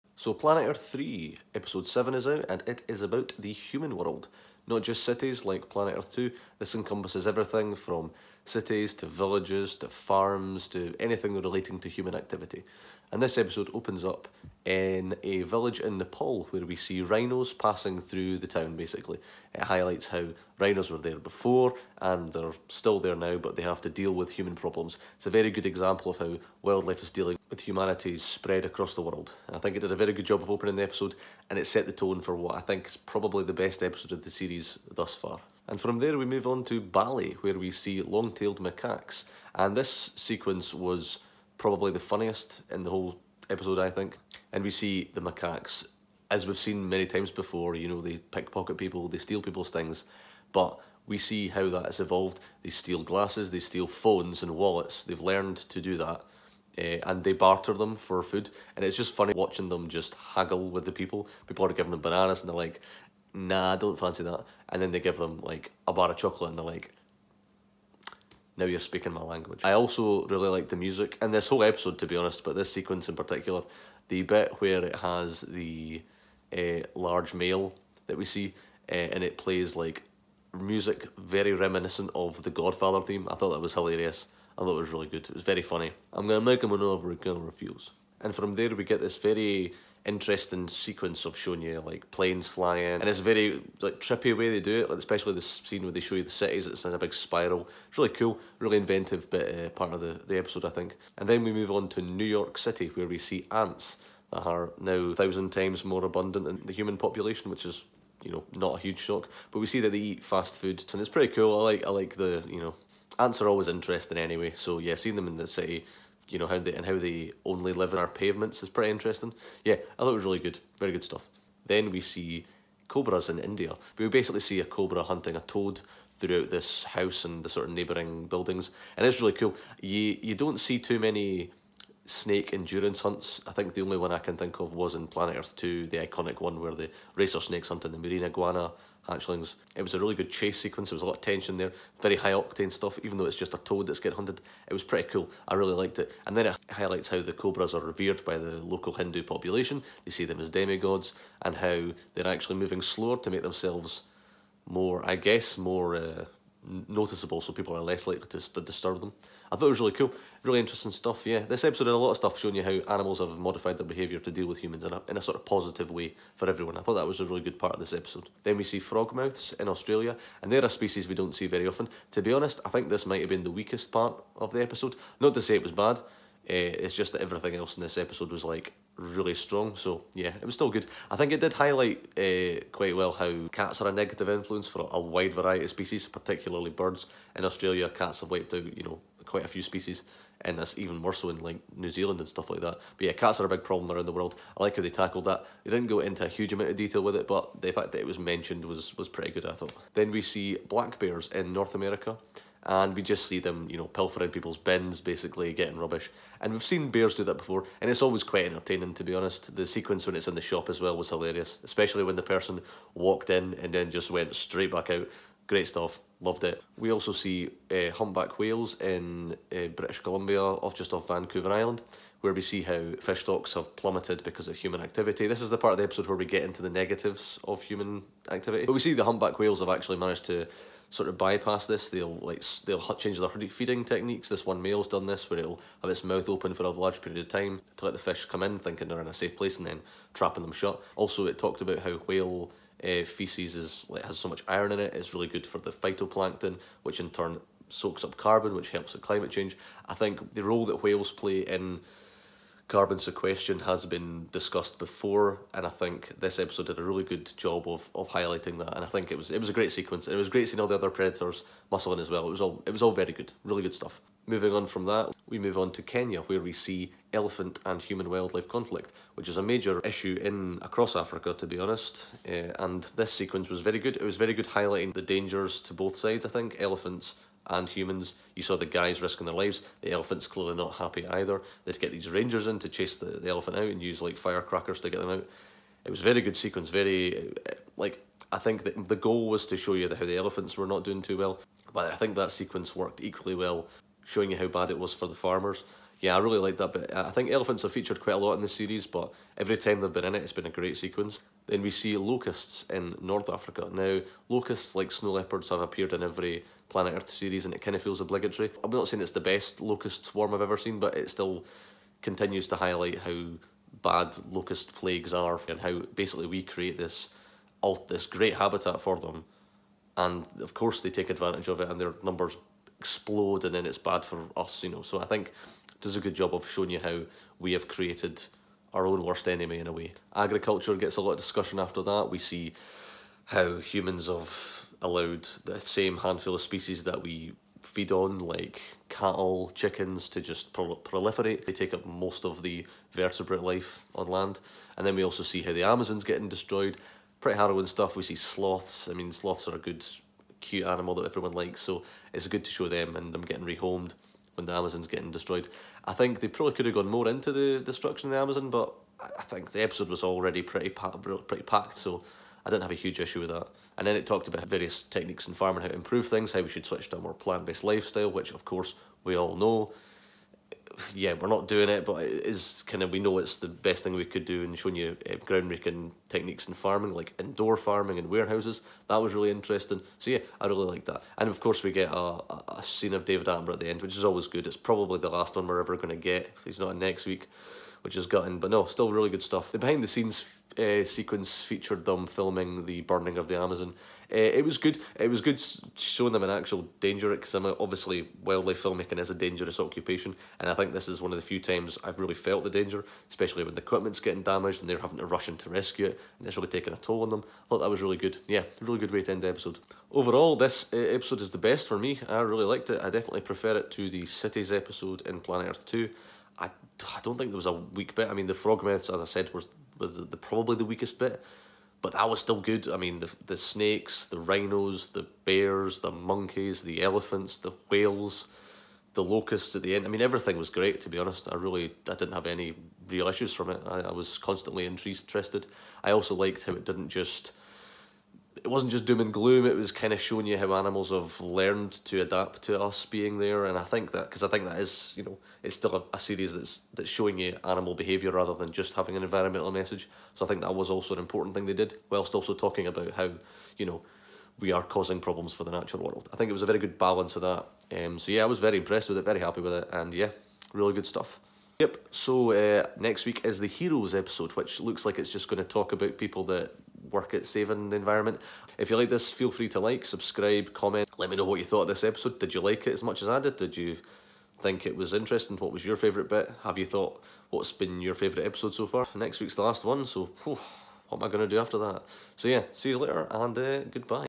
Planet Earth III: Episode 7- Human Review
In the penultimate episode of Planet Earth III we see the macaque mafia, hot dog-loving ants and whales that have invented a new way of catching fish. Only one more to go!